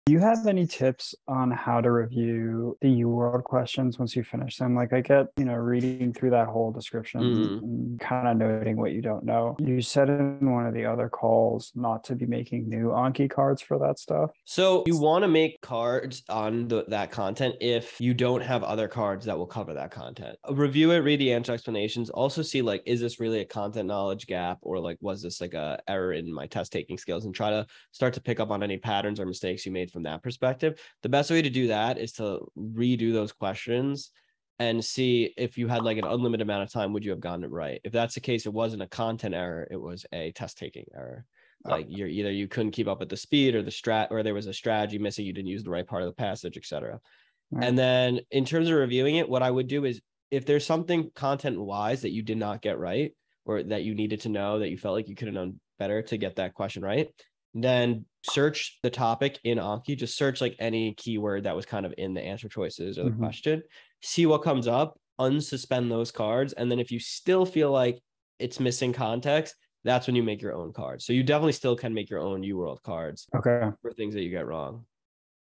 How To Review UWORLD Questions
If you were using uworld to study for the MCAT, it is important to not forget the material that you have studied. In my opinion, one of the best ways to do this is using Anki as I have seen this work for many students.

In order to save time, you can use one of the premade decks and unsuspend the content related to the question you got wrong. This is important because you now understand the context behind that content.

However, if a premade deck does not have the content or you feel you need more contacts. This is a good opportunity to make your own cards.

Interested in premed coaching to help you get into medical school with less stress?
Follow The Premed Consultants for on-the-go premed advice!